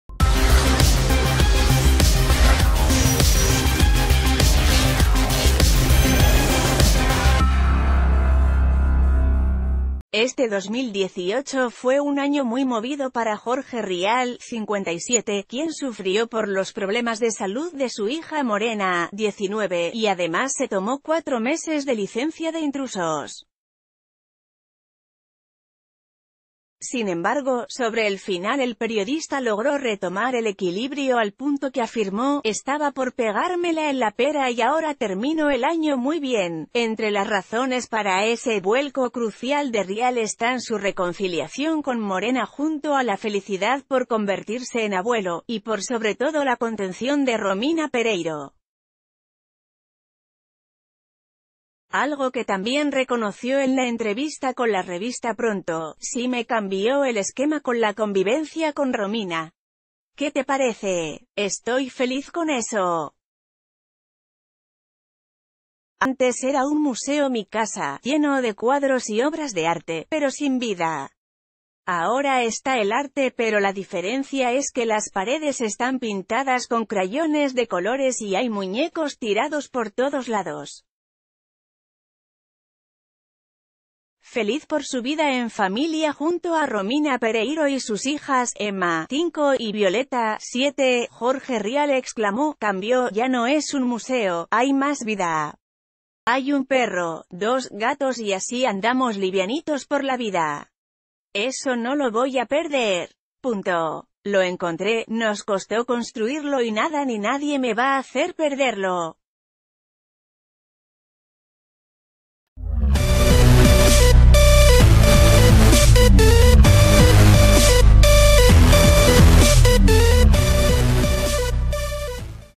Jorge Rial y el drástico cambio que implementó en su hogar: "Antes mi casa era..."
Este 2018 fue un año muy movido para Jorge Rial (57), quien sufrió por los problemas de salud de su hija Morena (19) y además se tomó cuatro meses de licencia de Intrusos. Sin embargo, sobre el final el periodista logró retomar el equilibrio al punto que afirmó: "Estaba por pegármela en la pera y ah...